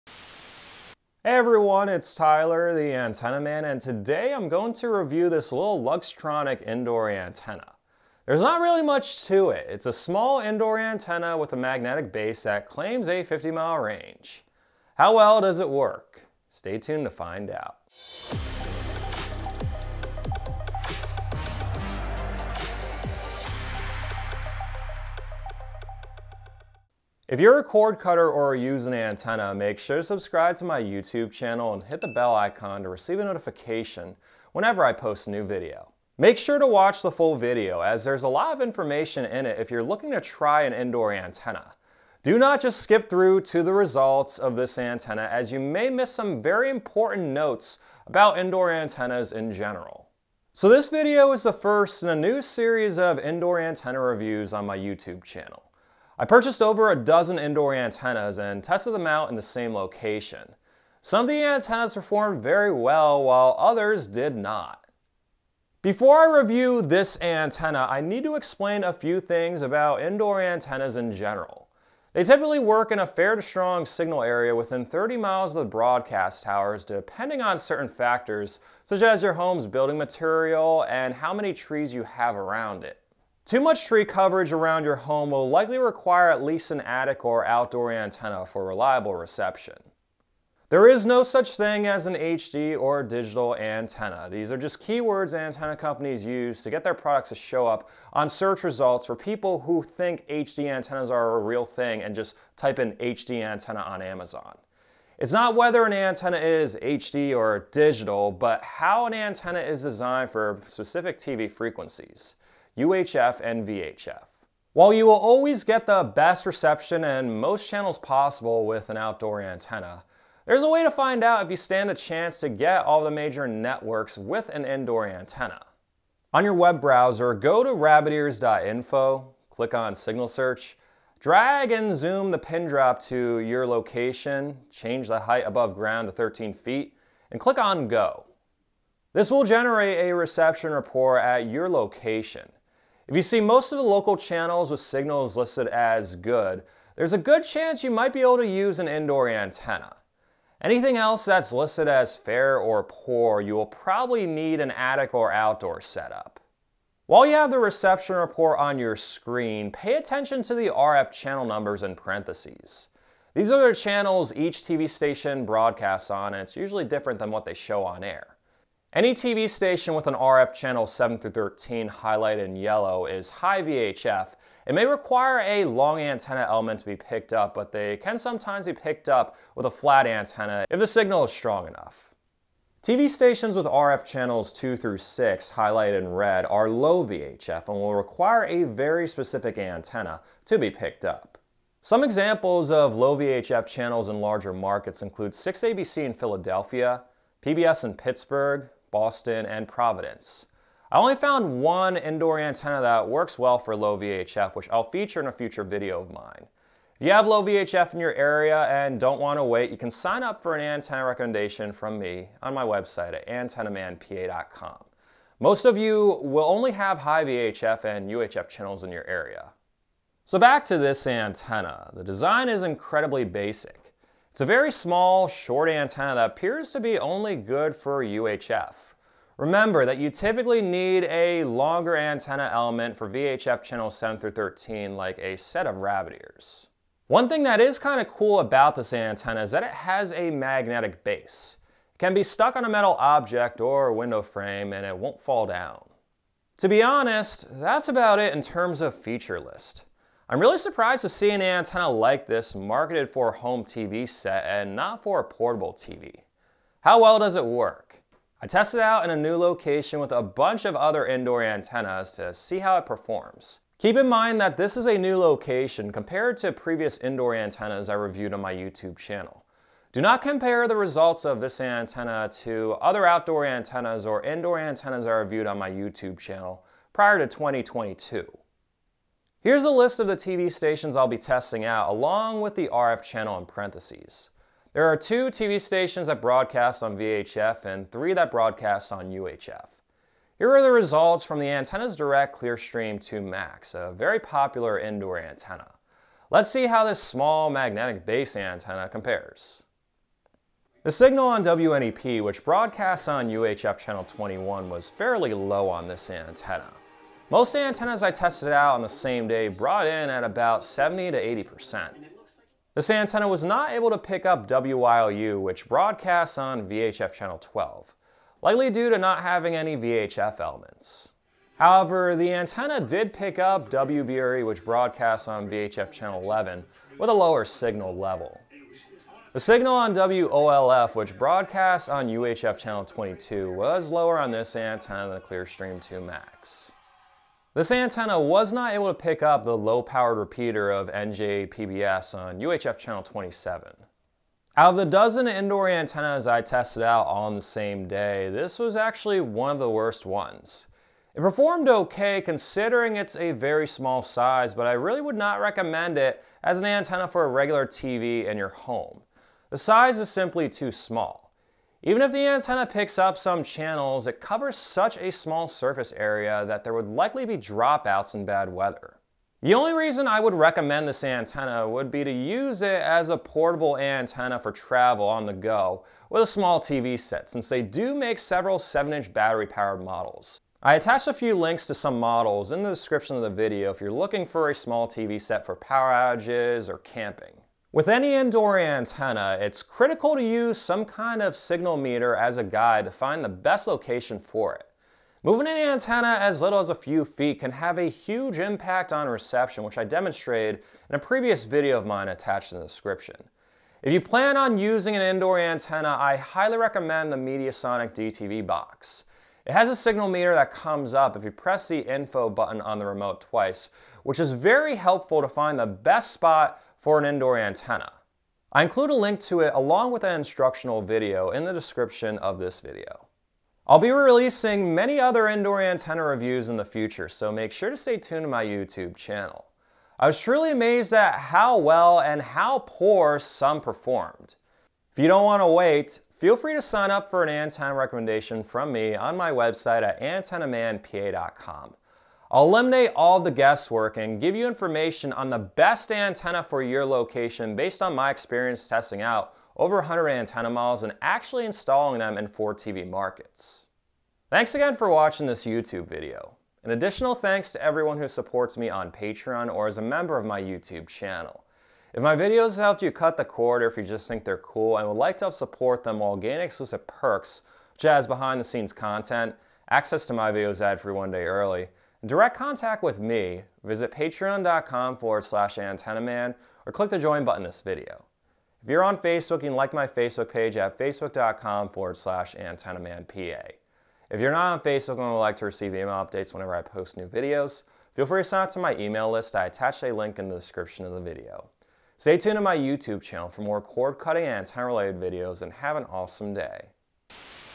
Luxtronic Magnetic Indoor "HD Digital" TV Antenna Review
In this video I review the following antenna: "Digital HDTV Antenna - Includes Magnetic Base and Coaxial Cable - 50 Mile Range - Indoor or Outdoor." It costs less than $20 on Amazon and claims a 50 mile range. How well does it actually work?

👇 Link to the antenna on Amazon

👇 Link to DTV box with signal meter & instructional video

👇 Link to 7" TV sets